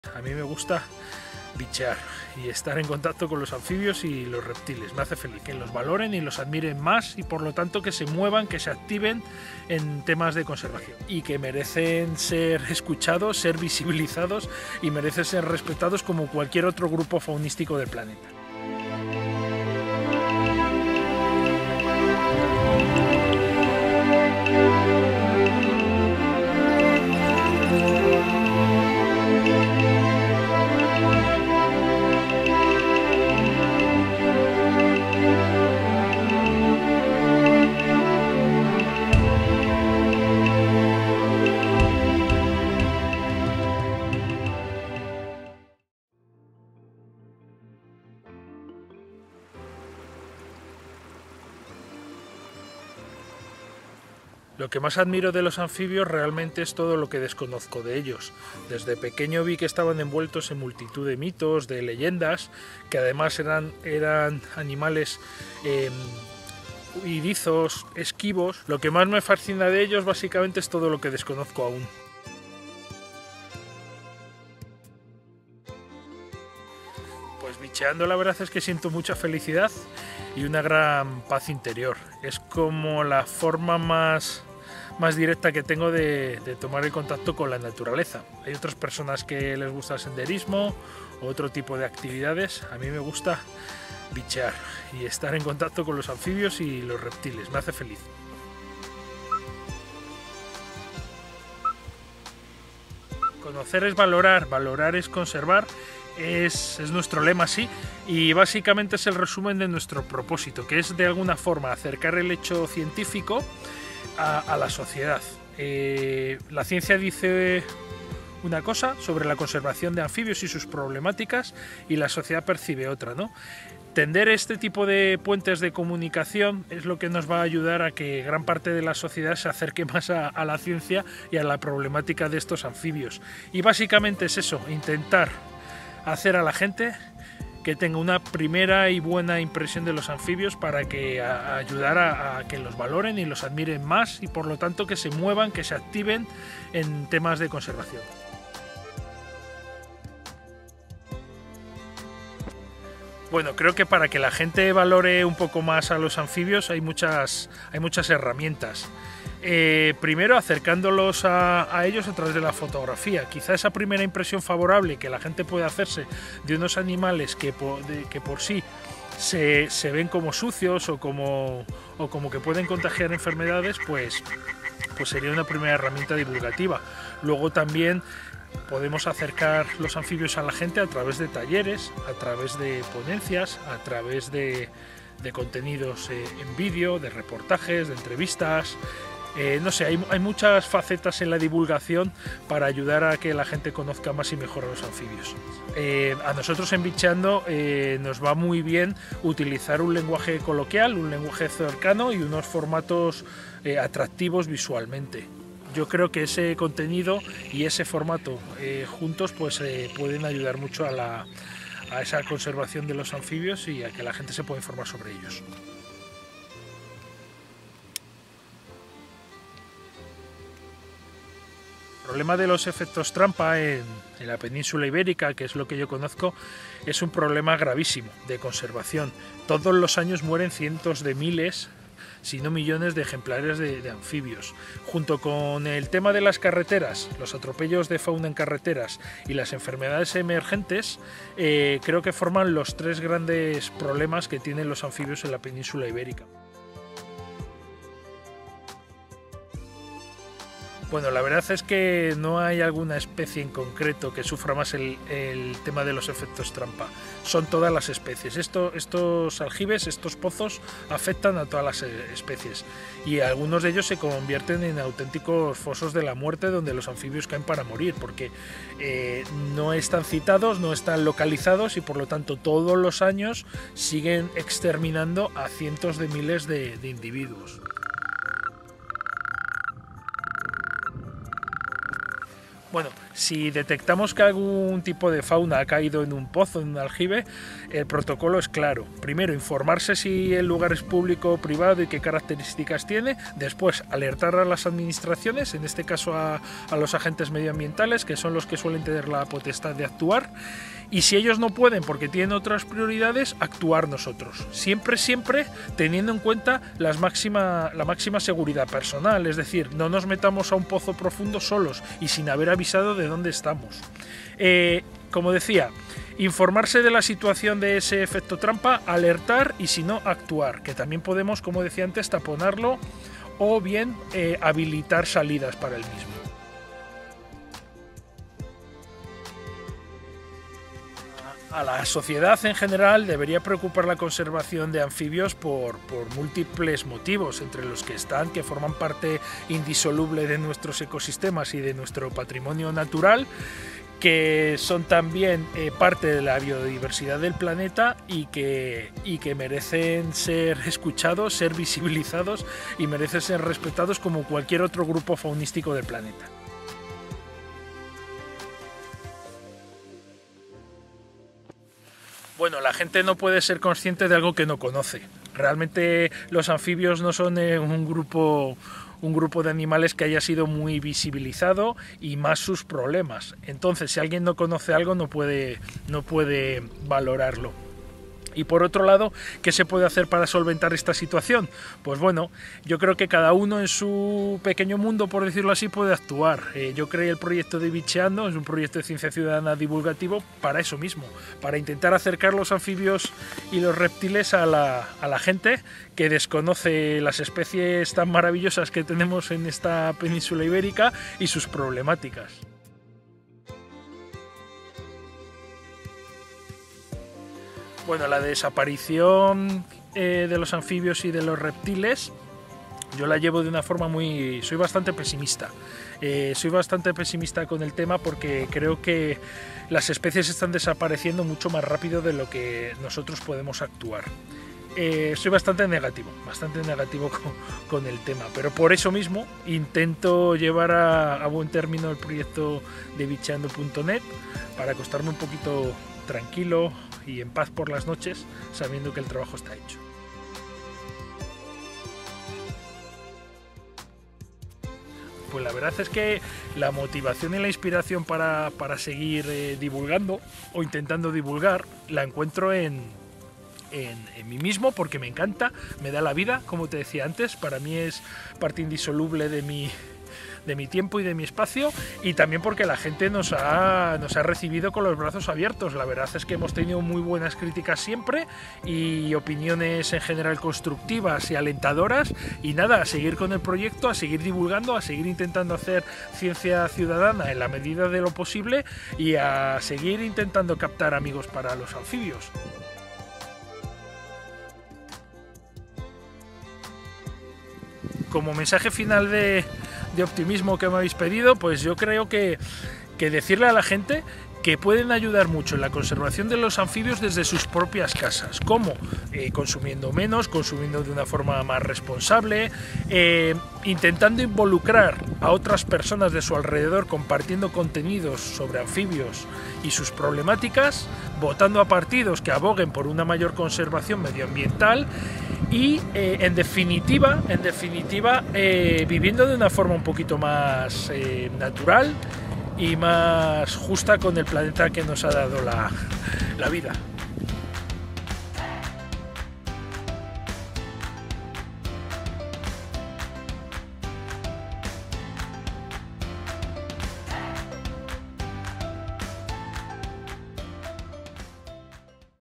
La Entrevista de Alberto González de Bicheando | El Documental El Valor de los Anfibios
Muchas gracias por tu trabajo Alberto y por participar en este pequeño-gran proyecto que trata de acercar los anfibios a las personas y honrar su valor :)

CONOCE A ALBERTO

Alberto González Divulgador en herpetología, guía herpetológico, docente, naturalista y fundador del proyecto Bicheando.net

CONTACTA CON ALBERTO